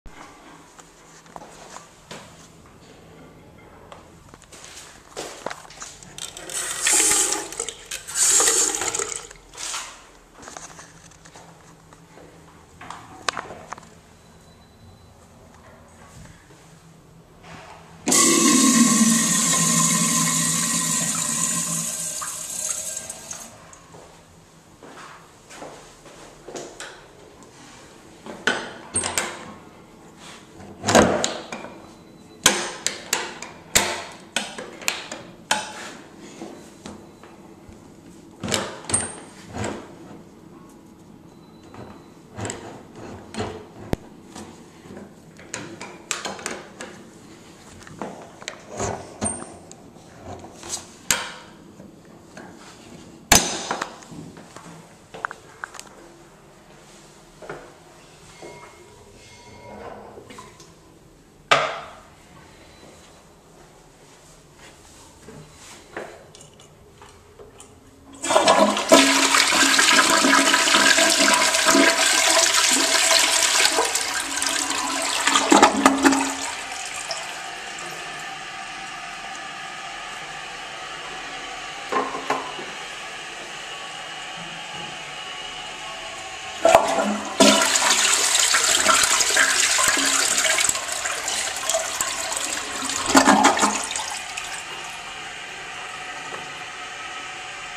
Old American Standard Washbrook urinal and 2011 Crane Plumbing Galaxy toilet
At Mexican restaurant. Video shot December 19, 2011